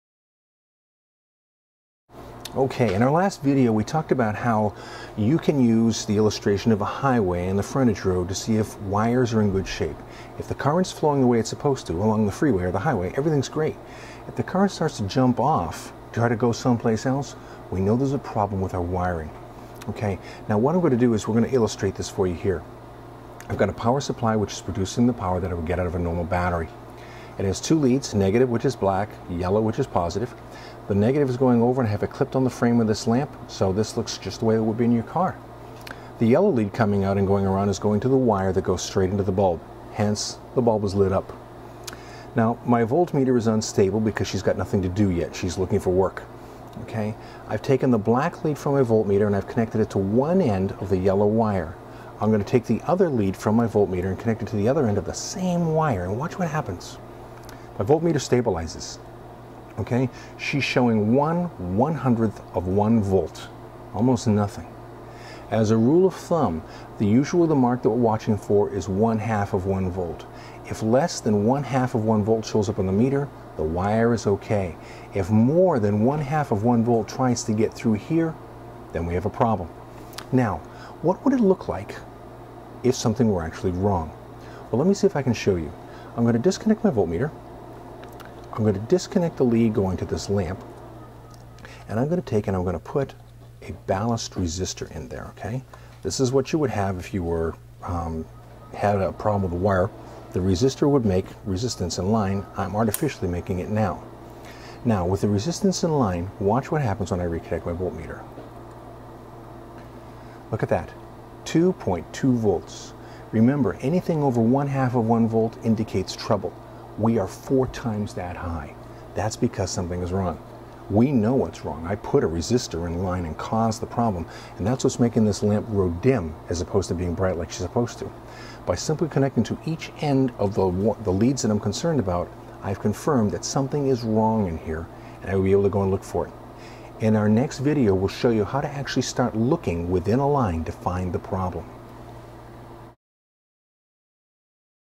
Voltage Drop (Pt 2) - Demonstration of How to Test
Here is a demonstration and explanation of the voltage drop method for testing continuity and diagnosing electrical problems.